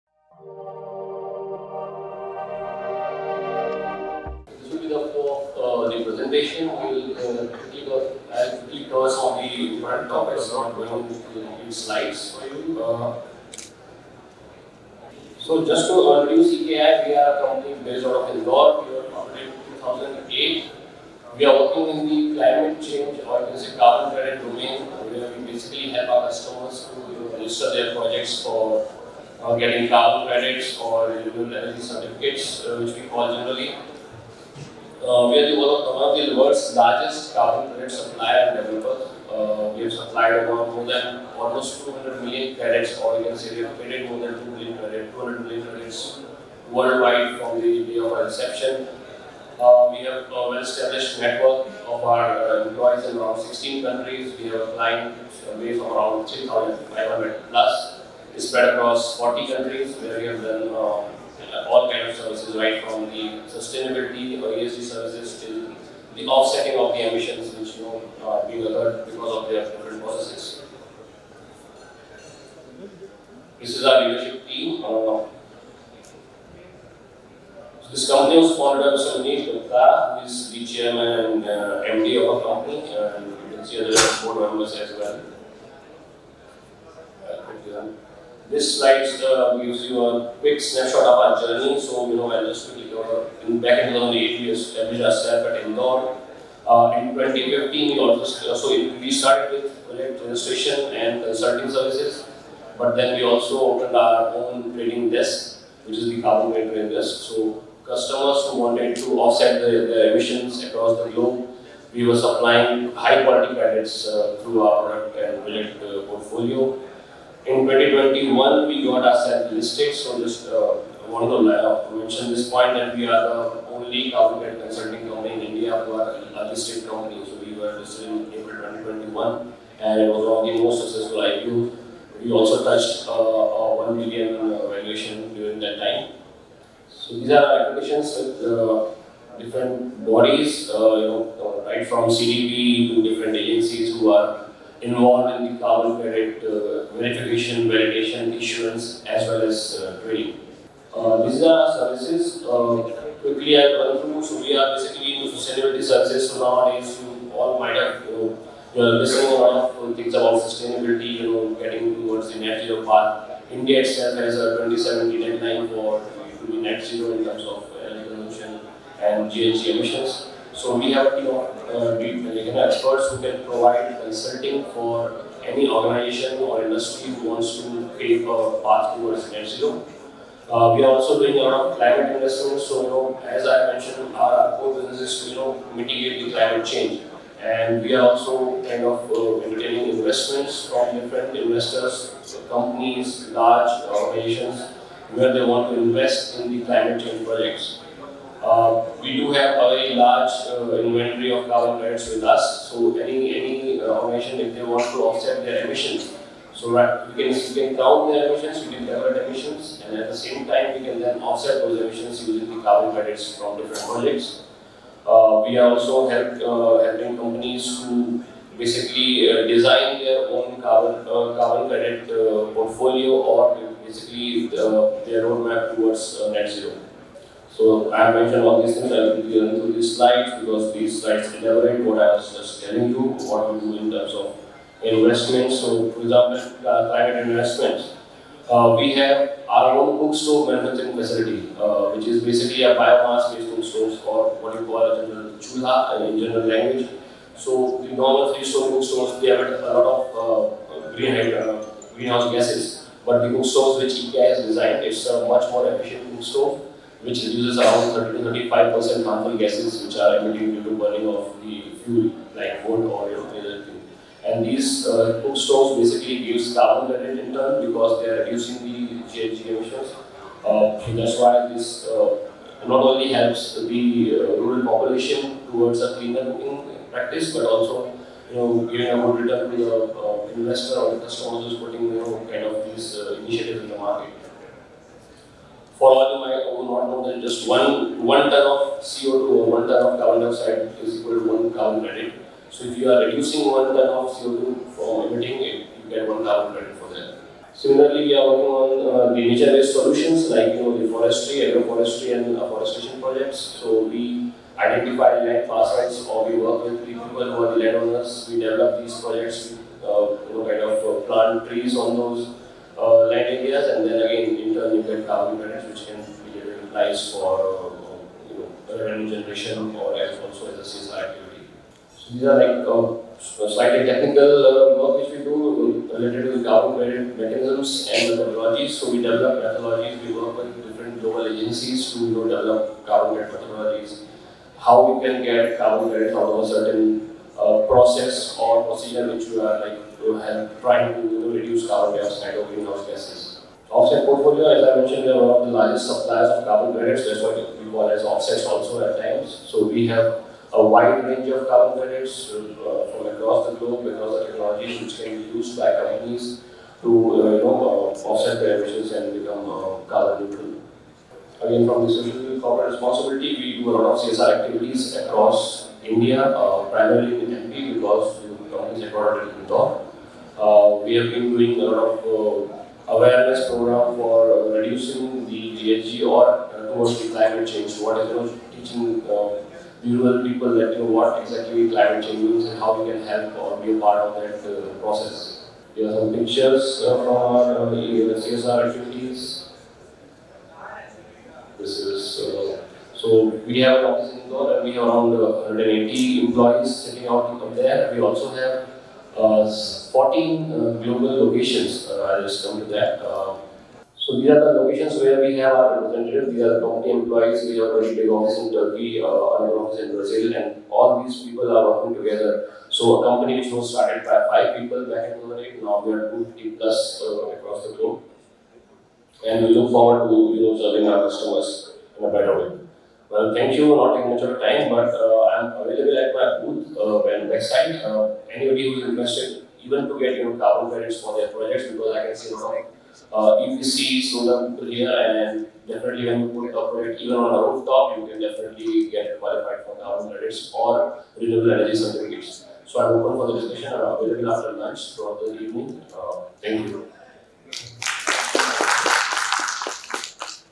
Presentation By Mr. Bhuwan Shukla, Deputy General Manager – Business Development – EKI Energy
Presentation By Mr. Bhuwan Shukla, Deputy General Manager – Business Development – EKI Energy Services Ltd. at PUNE SuryaCon Conference 2023